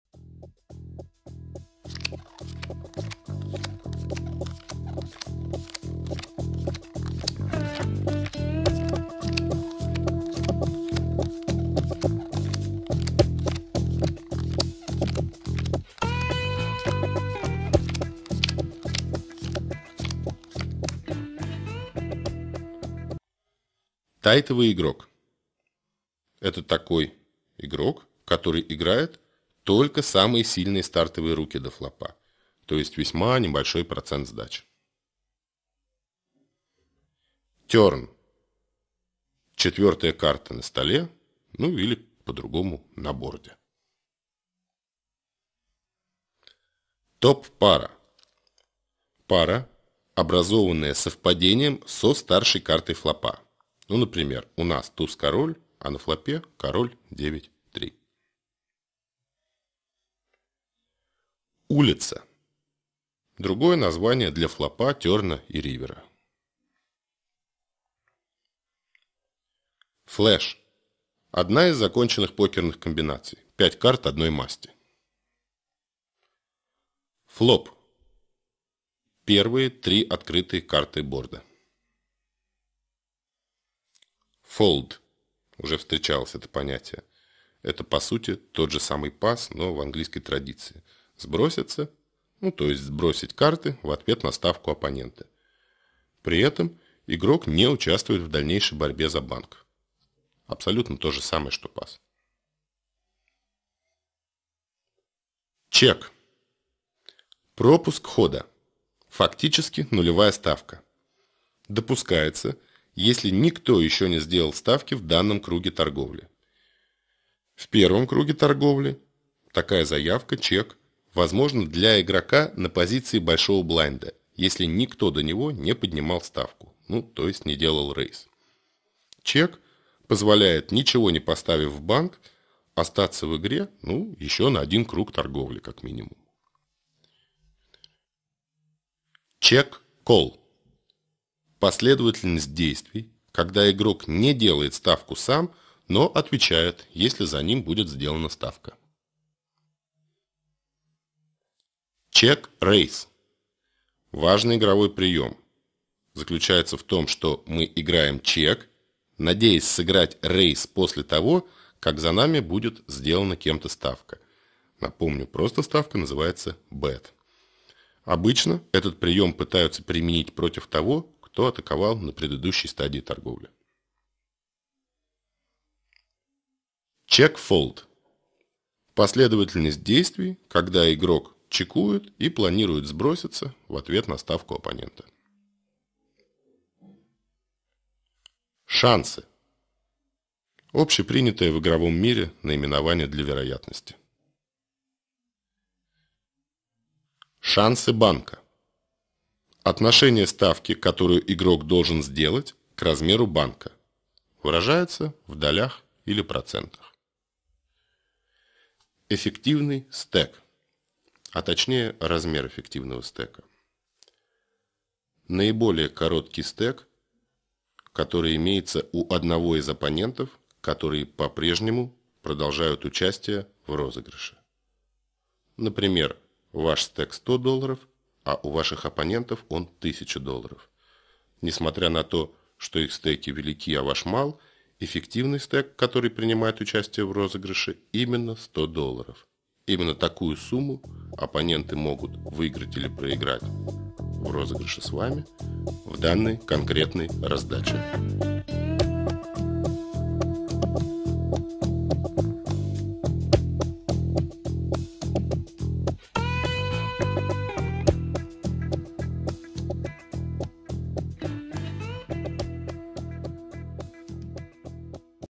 Р.ШАПОШНИКОВ - Школа онлайн. Занятие 3-е. Урок 6-й. "Глоссарий от Т до Э"
Тайтовый игрок. Тёрн. Топ-пара. Улица. Флеш. Флоп. Чек. Чек-колл. Чек-рейз. Чек-фолд. Шансы. Шансы банка. Эффективный стек.